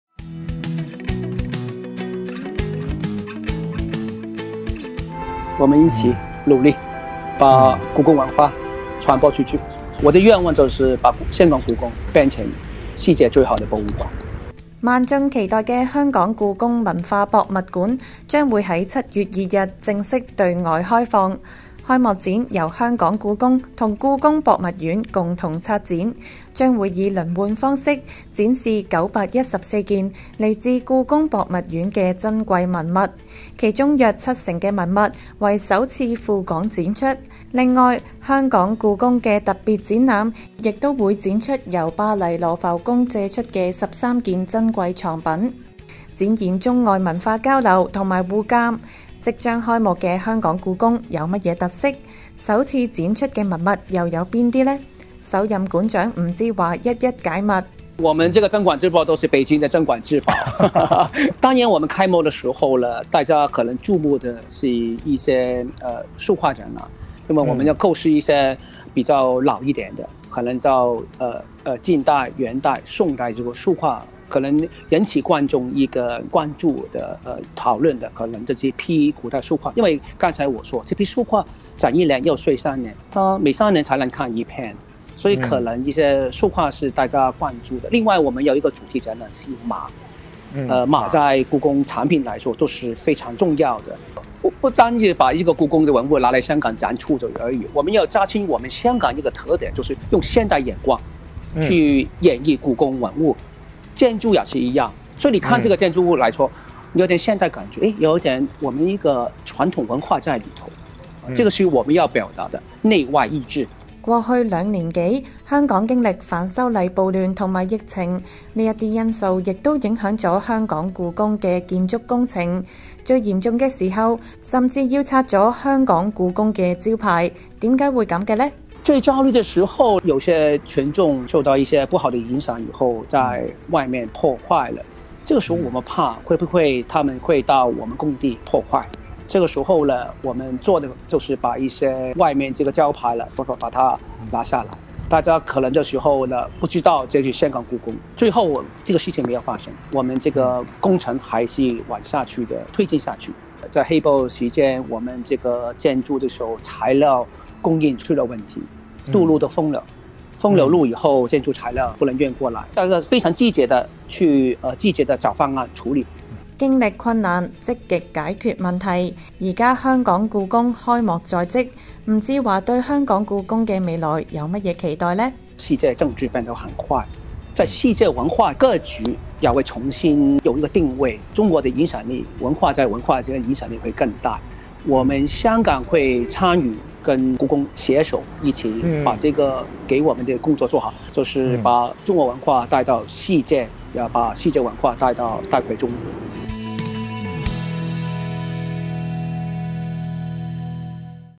【短片】【香港故宮即將開幕】有何鎮館之寶？暴亂影響施工？館長吳志華揭開幕後秘聞
//香港故宮文化博物館將於7月2日正式對外開放，首次展覽嘅藏品有咩特點？建造期間有咩不為人知的故事？一齊去片聽吳館長講講。//